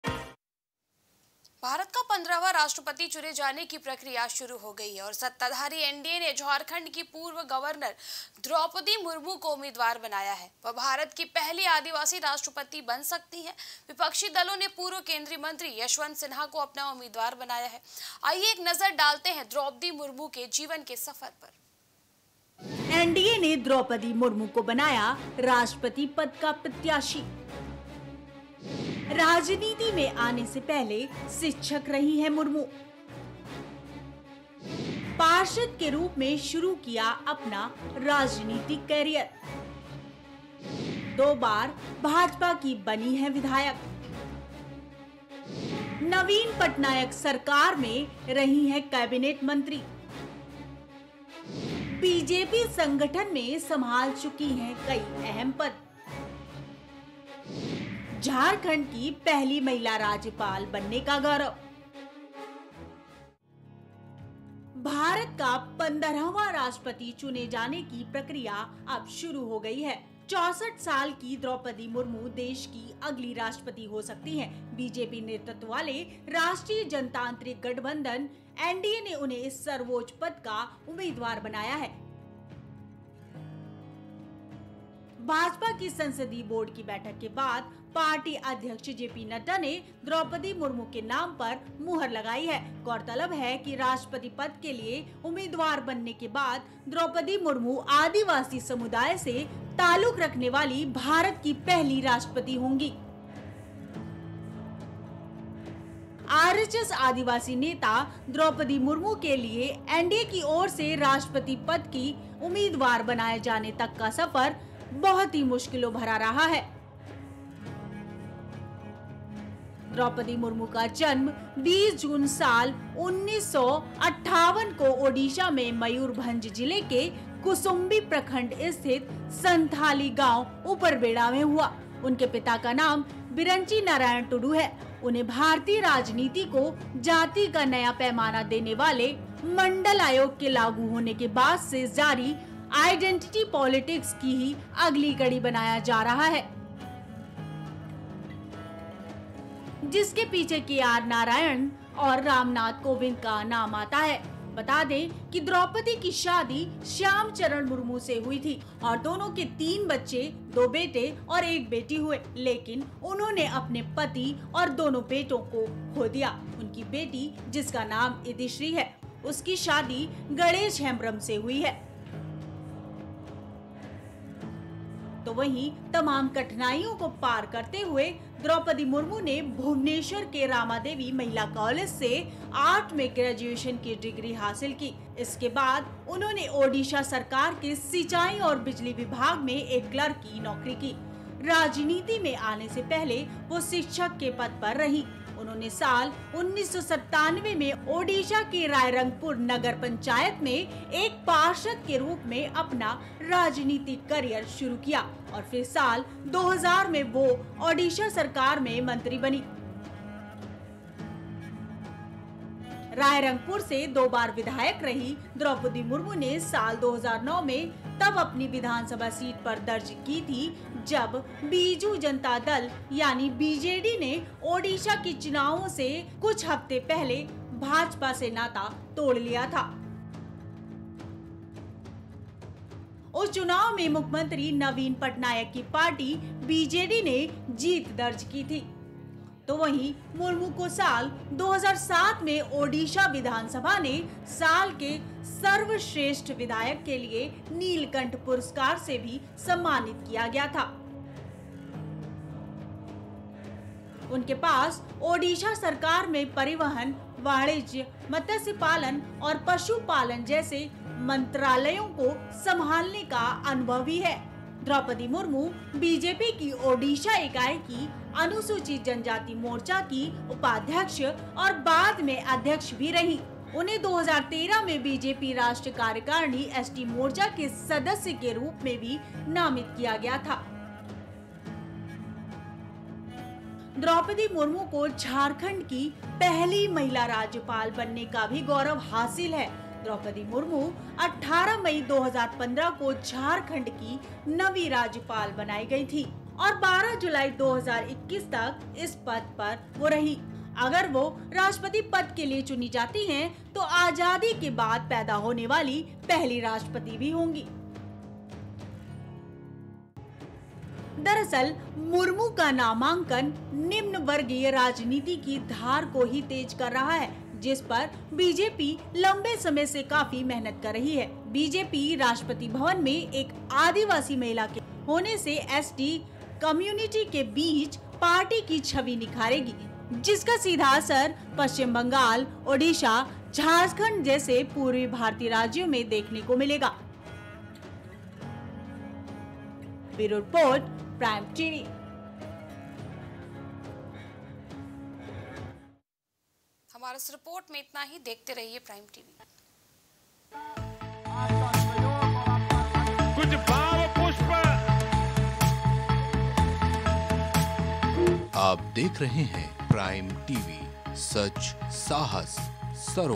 Presidential Election 2022 : जानिए कौन है Draupadi Murmu राष्ट्रपति पद की उम्मीदवार || Prime Tv
एनडीए ने द्रौपदी मुर्मू को बनाया राष्ट्रपति पद का प्रत्याशी
राजनीति में आने से पहले शिक्षक रहीं मुर्मू
पार्षद के रूप में शुरू किया अपना राजनीतिक कैरियर
दो बार भाजपा की बनी विधायक
नवीन पटनायक सरकार में रहीं कैबिनेट मंत्री
बीजेपी संगठन में संभाल चुकी हैं कई अहम पद
झारखंड की पहली महिला राज्यपाल बनने का गौरव